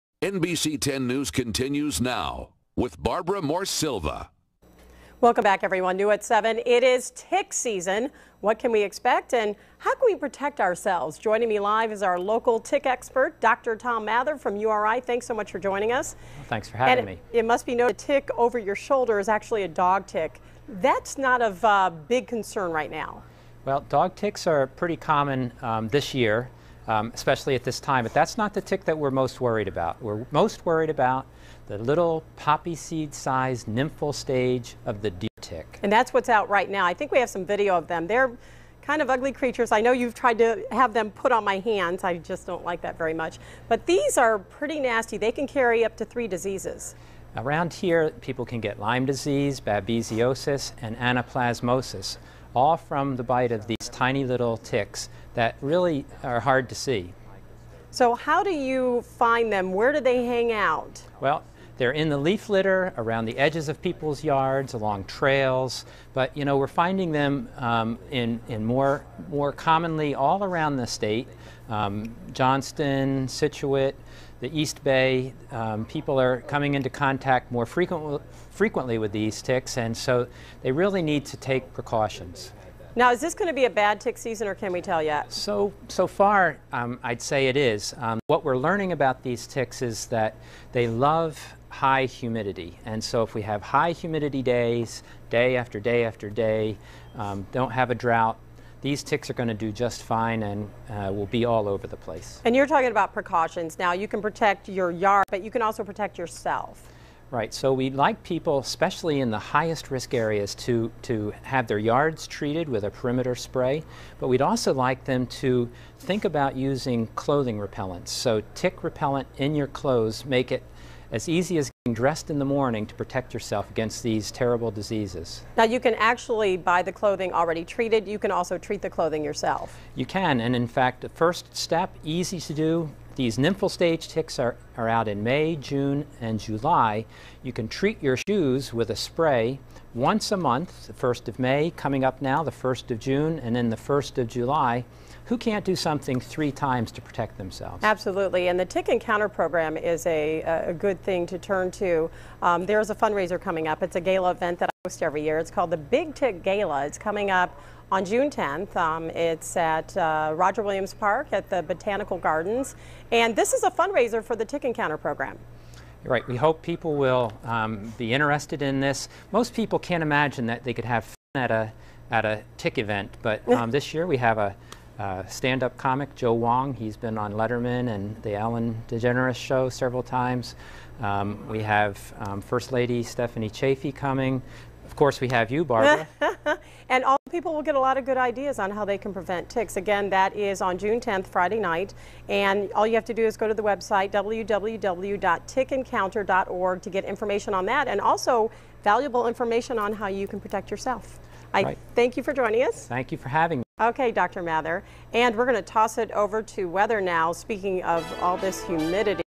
Tick Awareness Interview
NBC 10 Health Reporter Barbara Morse-Silva interviews Dr. Thomas Mather about tick awareness.